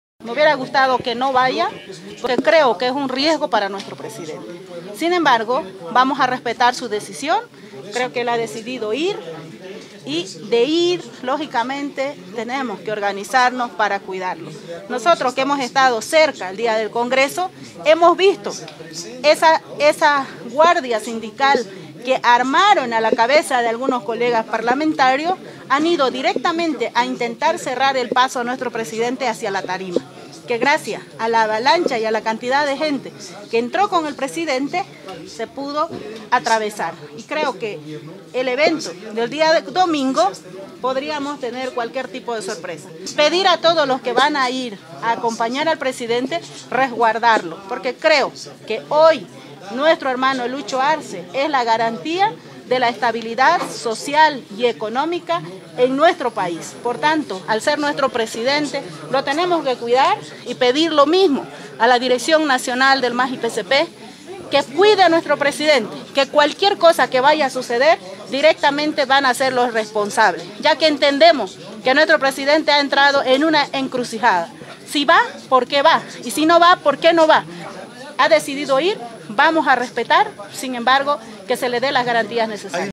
Renovadores piden garantías para Pdte. Arce en aniversario del M.A.S. La jefa de bancada del MAS
Renovadores piden garantías para Pdte. Arce en aniversario del M.A.S.

La jefa de bancada del MAS de Santa Cruz, Deysi Choque indico que le hubiese gustado que el Pdte Luis Arce no vaya al aniversario del MAS en Cochabamba porque es un riesgo contra el mandatario por ello pide garantías para su resguardo caso contrario responsabiliza a la direccion nacional por cualquier cosa que pueda pasar con la integridad del mandatario